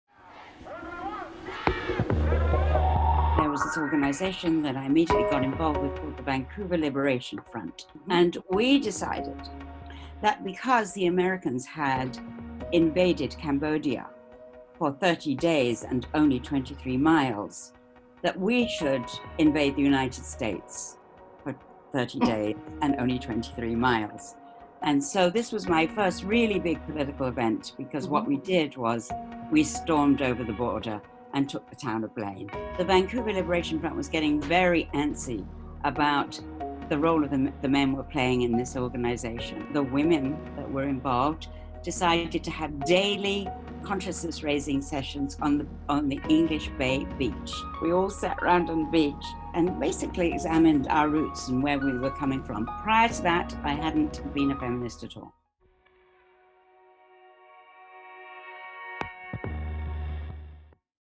Becoming Feminists: Sue Colley
A quick excerpt from Rise Up: Becoming Feminists. How some of the women in our collective gained feminist consciousness and became active in the women’s movement during the 1970s and 1980s.